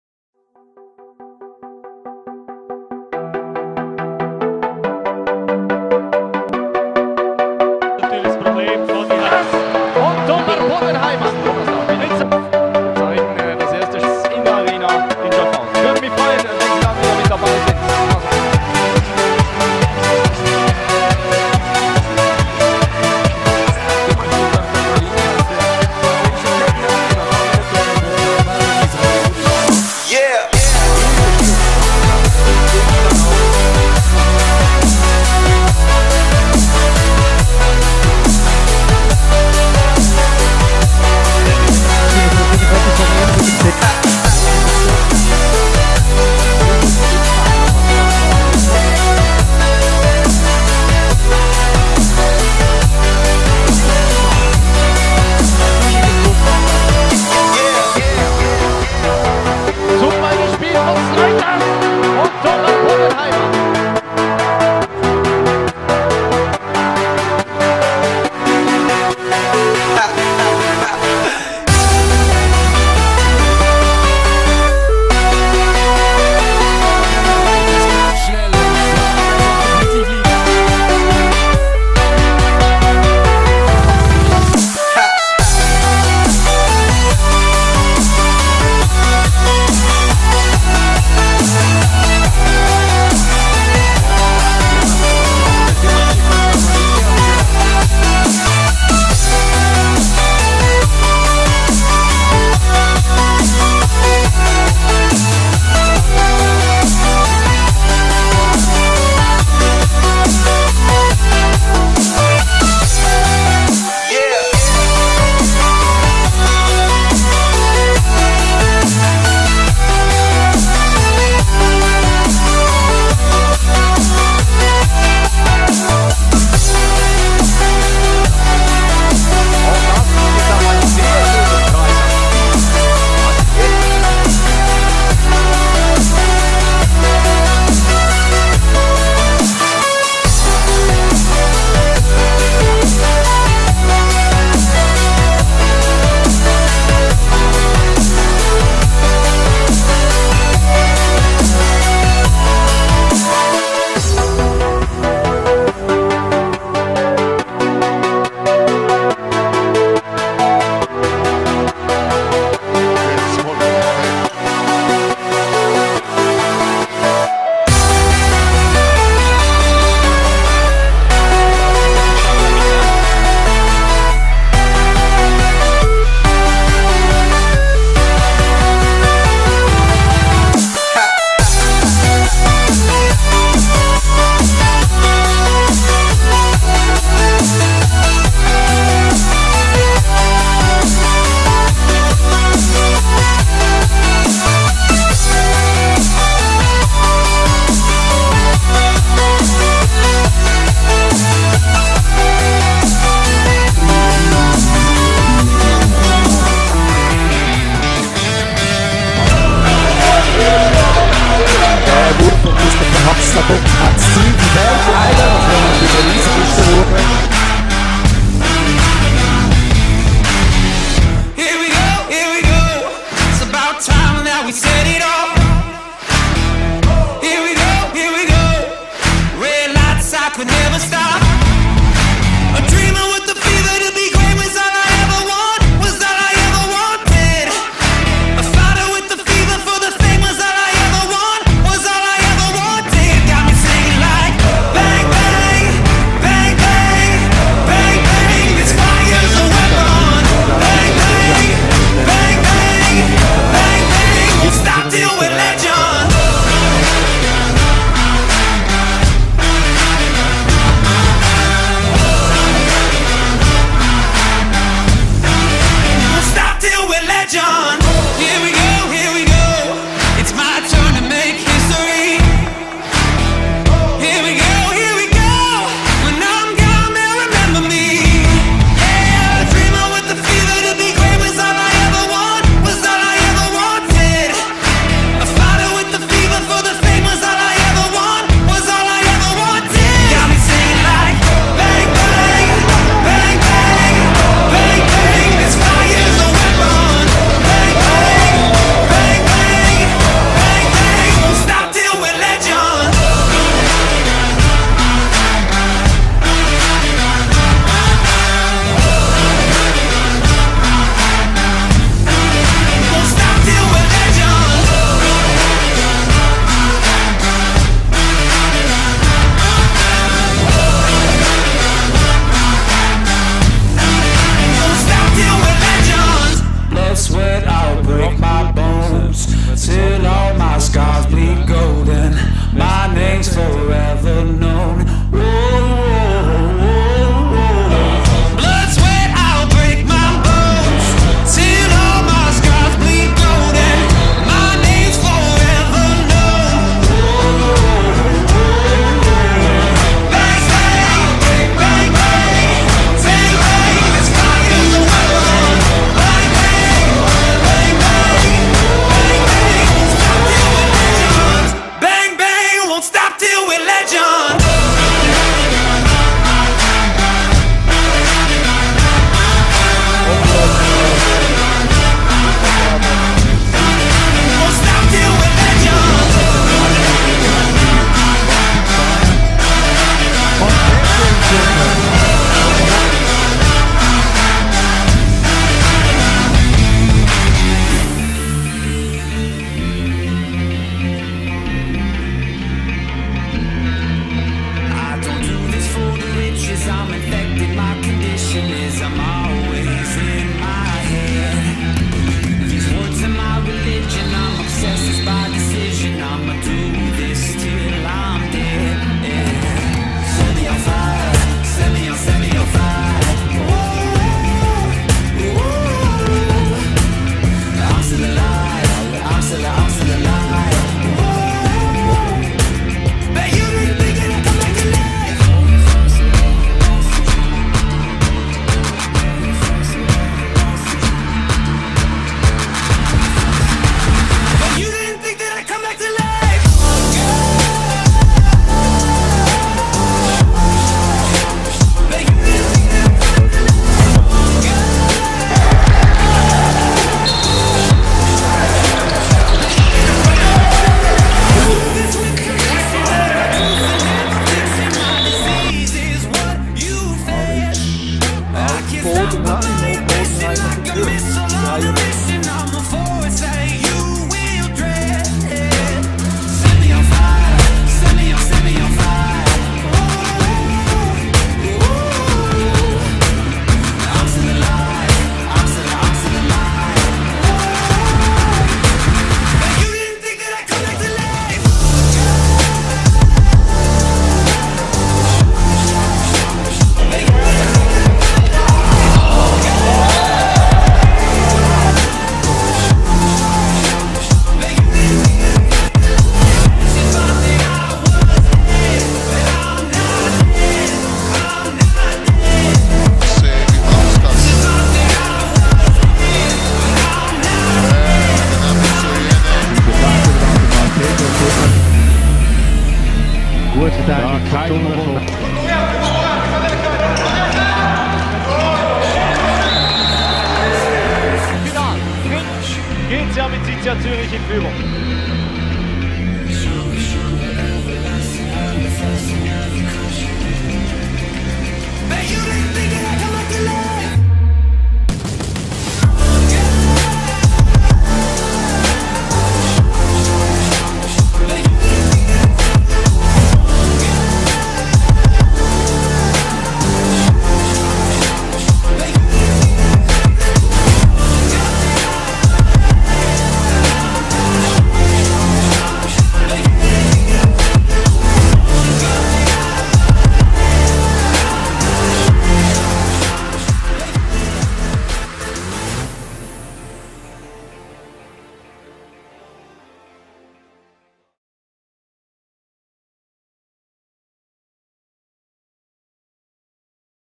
Tomer Bodenheimer - Season 2021/22 Highlights
Season Highlights of Tomer Bodenheimer, Handball player on the Pivot position for Israel National Team and for the 2022 Swiss Cup Winners, GC Amicitia Zurich.

Personal Details:
Tomer Bodenheimer
Position: Pivot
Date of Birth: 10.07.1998
Nationality: Israel & Switzerland 🇮🇱 🇨🇭
Height: 195 cm
Weight :101 kg